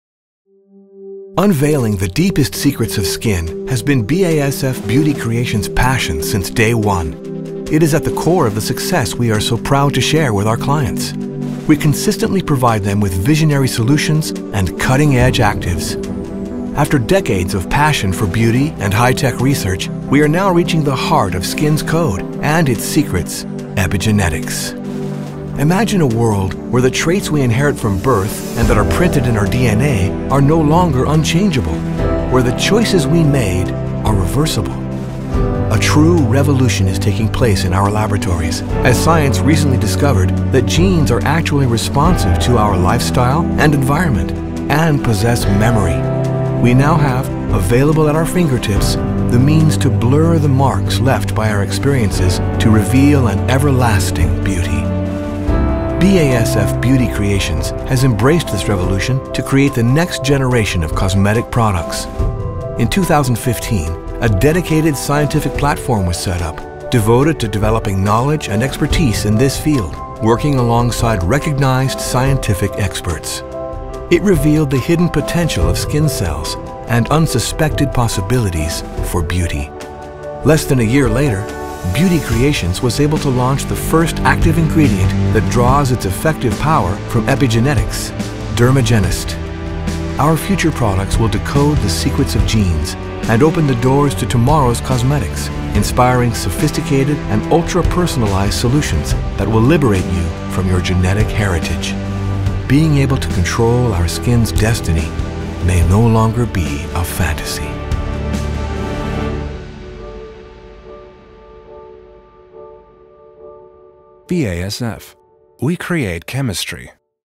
Innovation Platforms - Epigenetic
Building with nature’s building blocks: genetic research points to huge potential for ingredients that are capable of reprogramming skin cells.